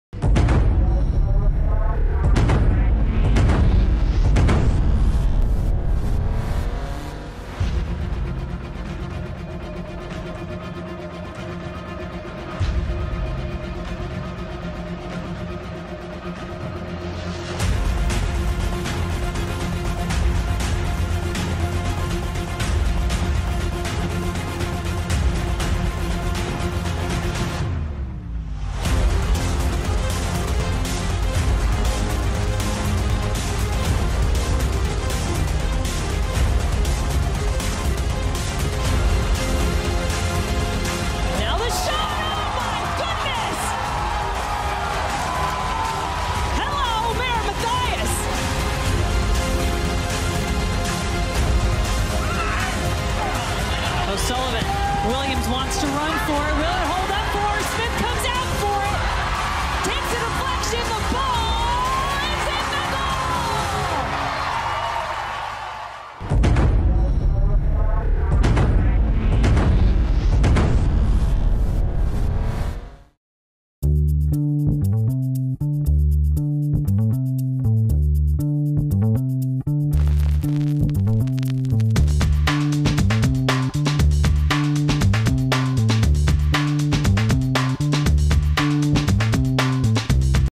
NC Courage vs Utah Royals - April 21st 2018 - Continental Recap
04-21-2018

Check out some of the highlights from field-side as we recap the April 21st 2018 match up against the Utah Royals!